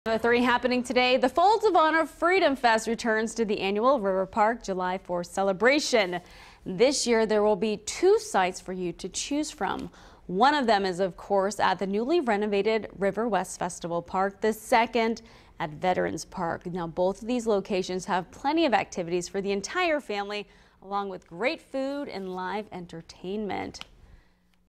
Freedom Fest returns to Tulsa
Need Fourth of July plans? Then come check out Freedom Fest for family fun and fireworks.